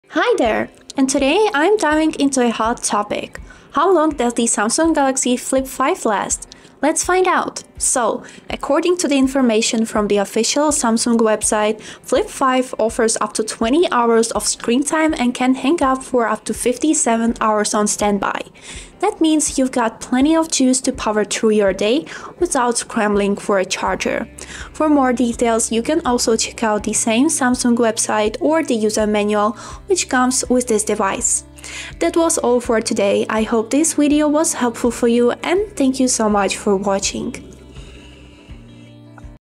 Samsung Galaxy Z Flip 5: How Long Does the Battery Really Last?
The Samsung Galaxy Z Flip 5 is not just a stylish foldable—it packs a punch with its innovative features. But how long does the battery last, and are you maximizing its potential? In this video, we delve into the secrets of the Z Flip 5's battery life. Get ready to unlock the full potential of your Galaxy Z Flip 5!